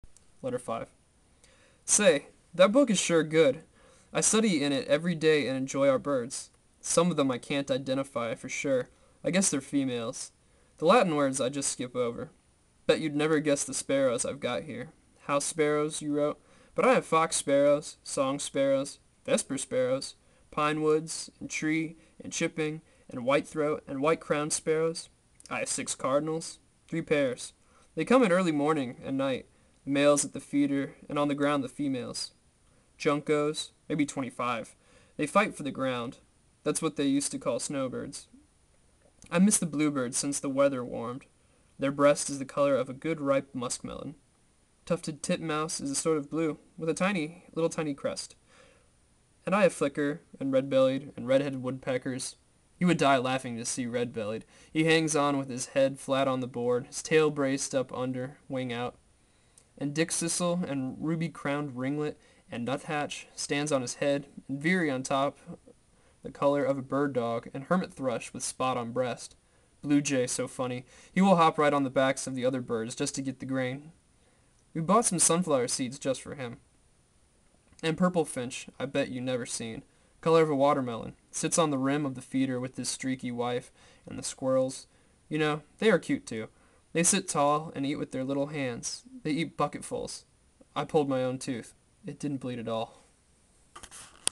Mona Van Duyn - Letters From a Father (5)
Mona van Duyn was born in 1921 in Waterloo, Iowa, and died in 2004.  Between 1974 and 1978 she and her husband edited "Perspective: A Quarterly of Literature."  She served as Poet Laureate of the United States from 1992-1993.

Letters From a Father

5

Say, that book is sure good, I study
in it every day and enjoy our birds.
Some of them I can't identify
for sure, I guess they're females, the Latin words
I just skip over.  Bet you'd never guess
the sparrows I've got here, House Sparrows you wrote,
but I have Fox Sparrows, Song Sparrows, Vesper Sparrows,
Pine Woods and Tree and Chipping and White Throat
and White Crowned Sparrows.  I have six Cardinals,
three pairs, they come at early morning and night,
the males at the feeder and on the ground the females.
Juncos, maybe 25, they fight
for the ground, that's what they used to call snowbirds.  I miss
the Bluebirds since the weather warmed.  Their breast
is the color of a good ripe muskmelon.  Tufted Titmouse
is sort of blue with a little tiny crest.
And I have Flicker and Red-Bellied and Red-
Headed Woodpeckers, you would die laughing
to see Red-Bellied, he hangs on with his head
flat on the board, his tail braced up under,
wing out.  And Dickcissel and Ruby Crowned Ringlet
and Nuthatch stands on his head and Veery on top
the color of a bird dog and Hermit Thrush with spot
on breast, Blue Jay so funny, he will hop
right on the backs of the other birds to get the grain.
We bought some sunflower seeds just for him.
And Purple Finch I bet you never seen,
color of a watermelon, sits on the rim
of the feeder with his streaky wife, and the squirrels,
you know, they are cute too, they sit tall
and eat with their little hands, they eat bucketfuls.
I pulled my own tooth, it didn't bleed at all.